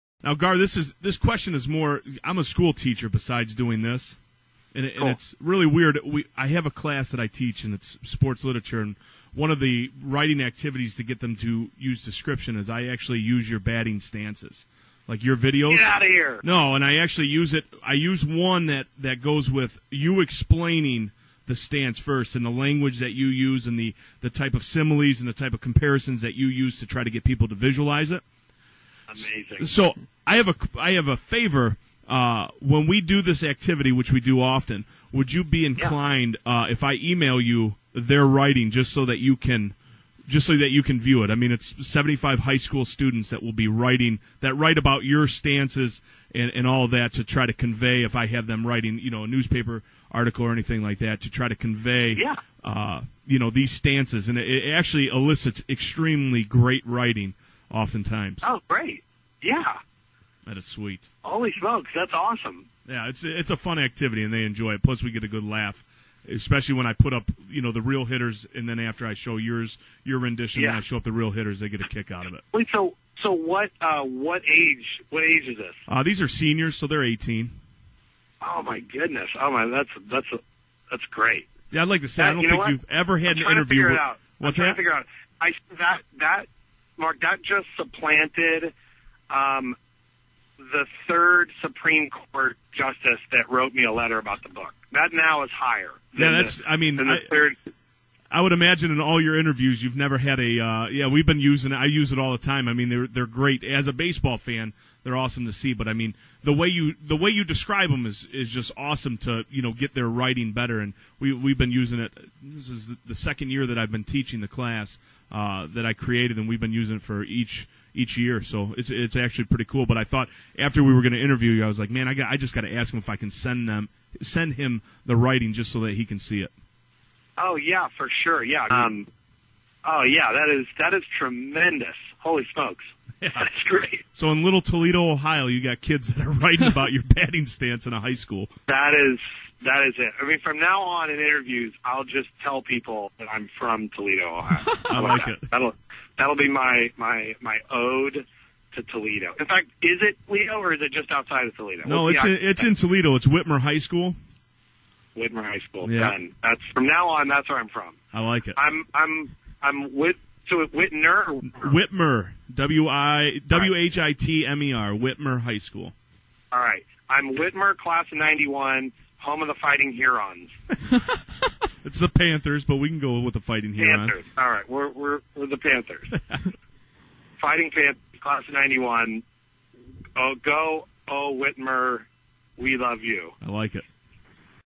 Batting Stance Guy interview clip
Gar Ryness joins Nate Lowe for an interview on WRSC Radio.com.